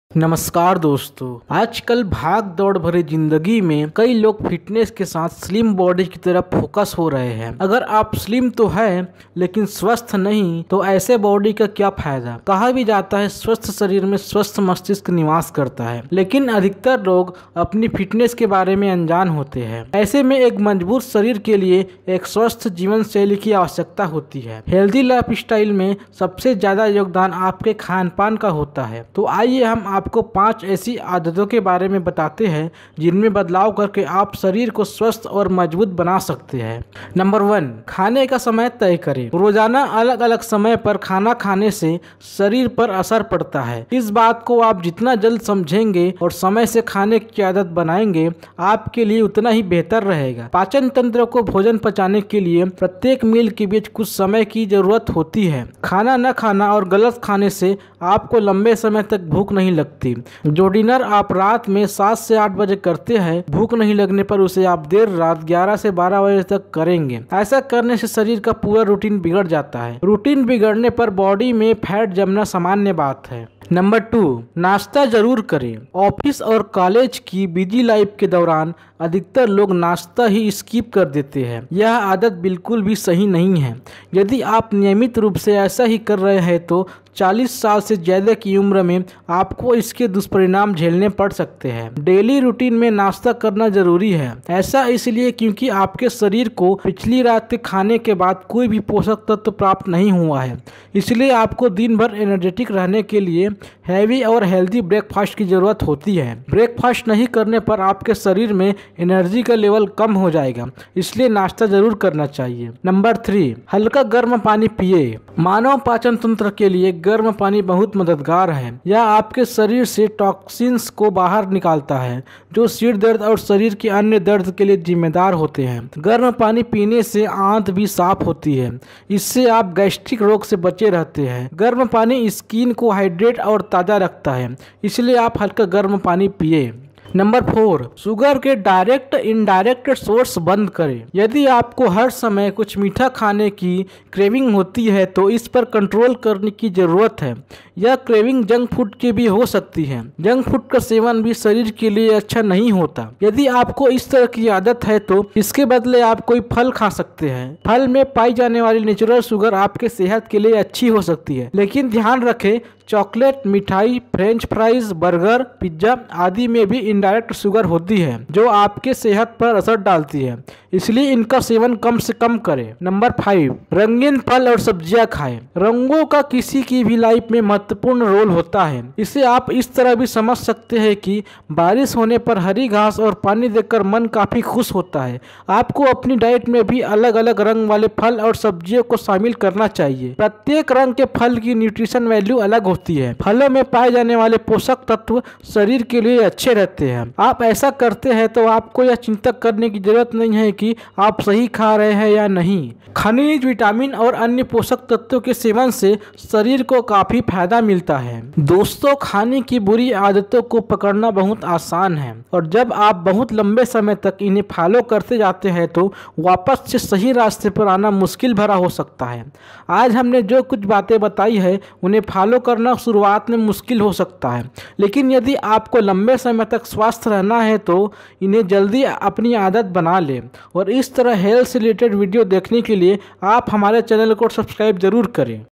खाने की ये 5 आदतें जरूर बदले | healthy lifestyle | Health Tips In Hindi
many people are focusing on slim body with fitness. If you are slim but not healthy, then what is the use of such a body? It is also said that healthy brain resides in a healthy body. But most people are unaware of their fitness.
In such a situation, a healthy body requires a healthy lifestyle. Your food contributes the most in healthylifestyle. So let us tell you about 5 such habits in which you can make the body healthy and strong by making changes

I AM THE NOT THE OWNER OFF ANY PICTURES SHOWN IN THE VIDEO )